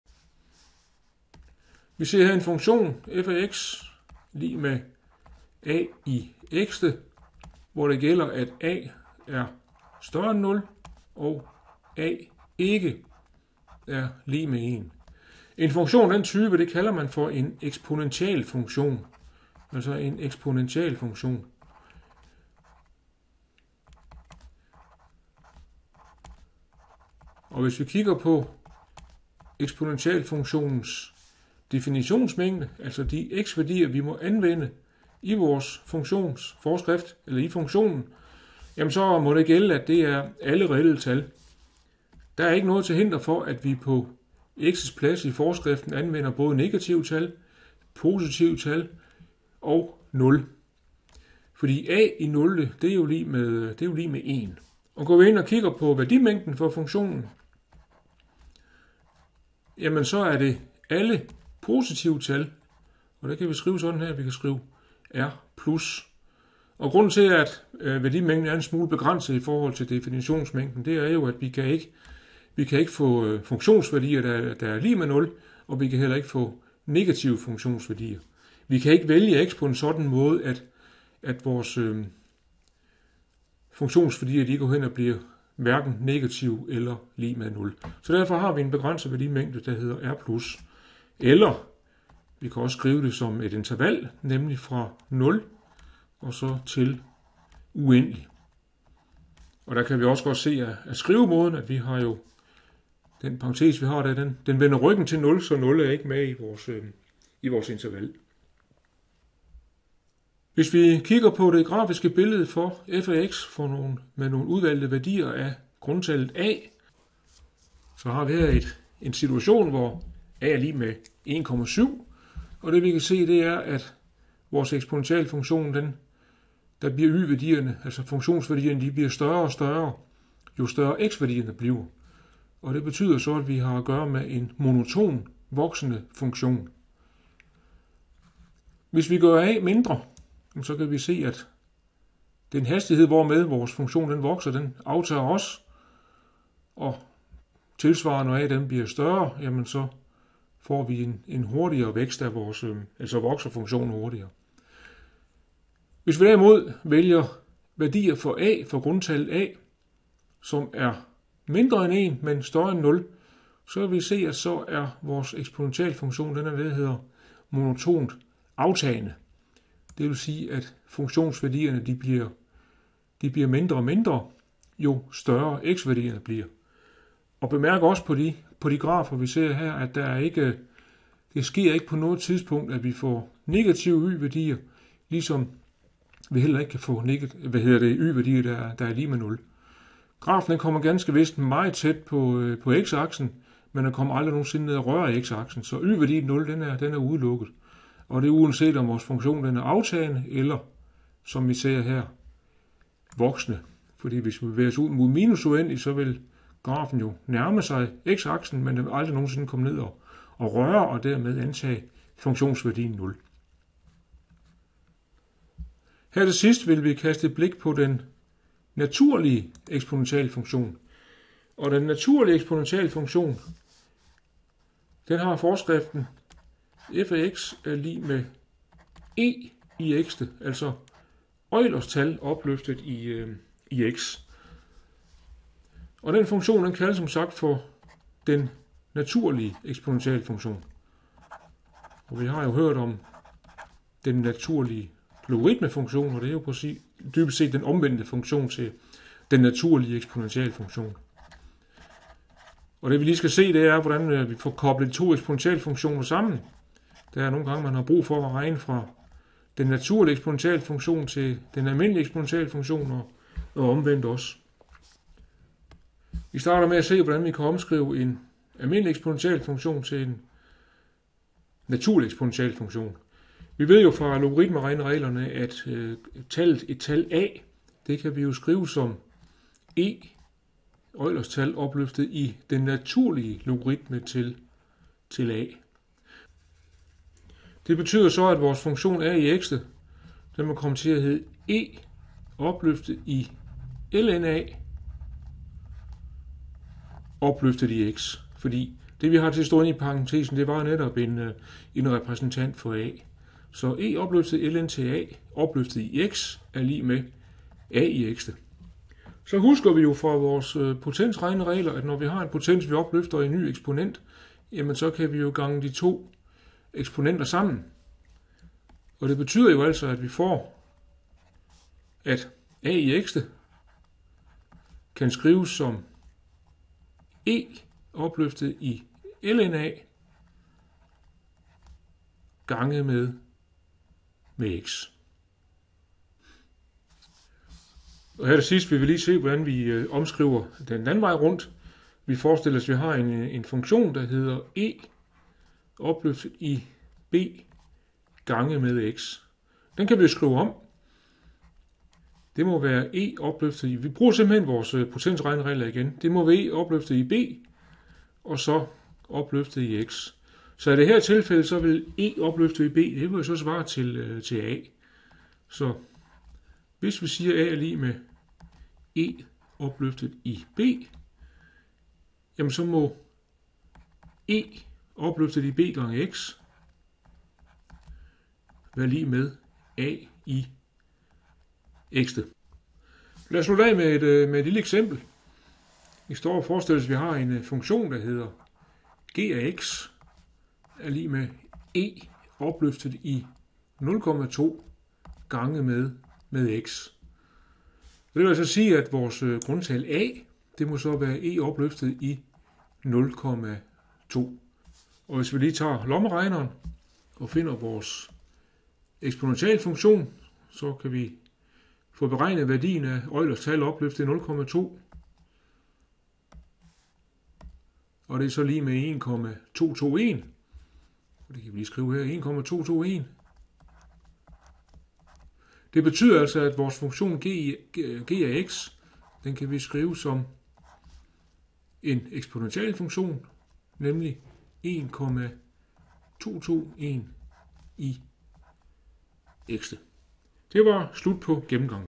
Den almindelige og den naturlige eksponentialfunktion
Gennemgang af den almindelige og den naturlige eksponentialfunktion